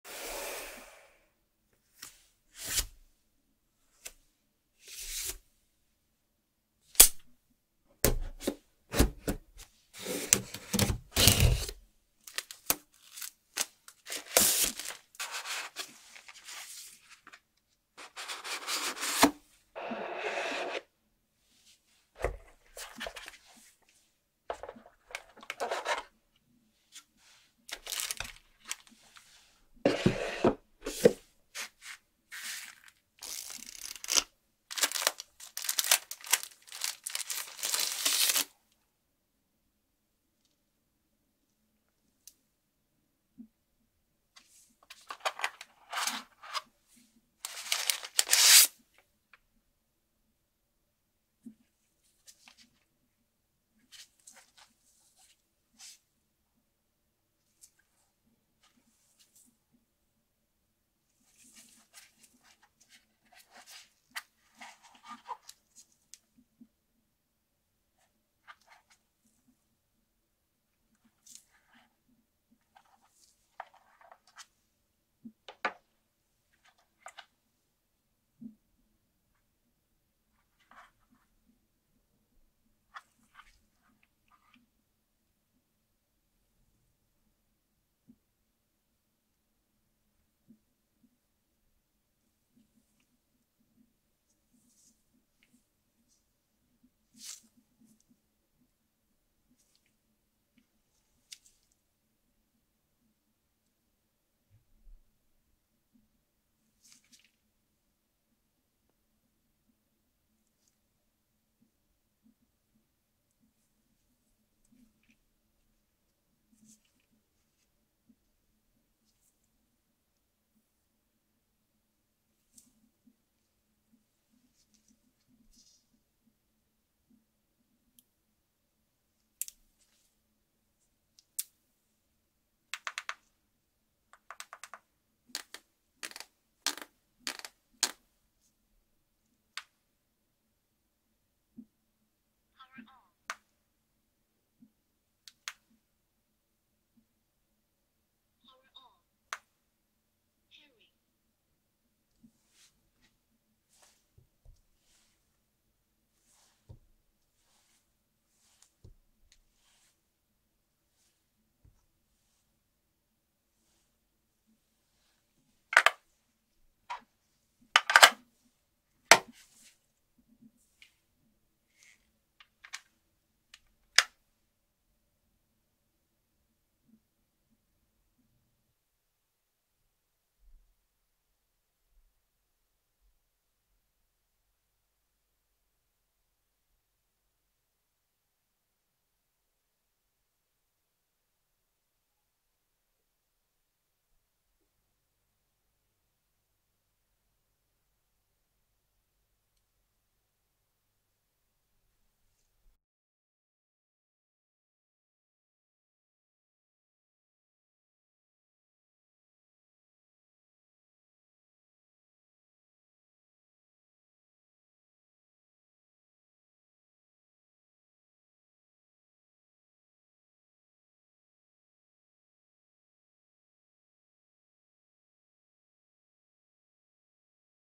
SIVGA Open Ear True Wireless Sports Earphone Unboxing - ASMR
Here we Xtract and unbox the SIVGA Open Ear True Wireless Sports Earphone